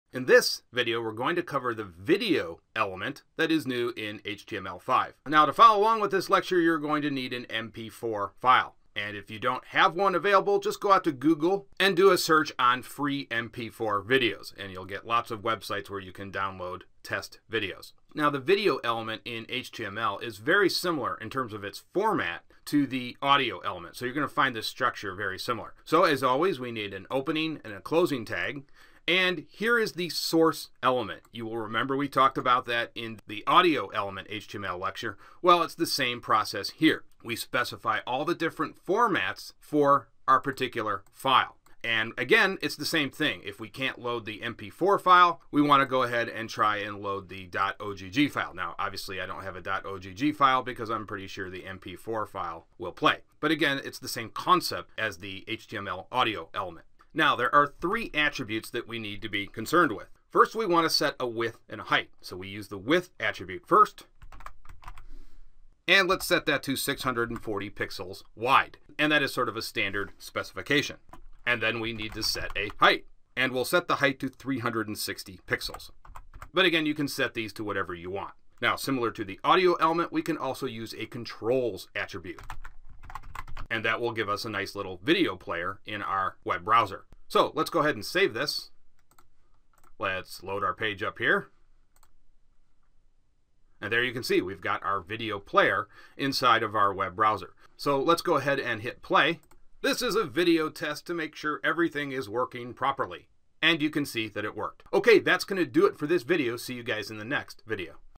HTML Tutorial for Beginners - 28 - Video element
In this video we go over the video element.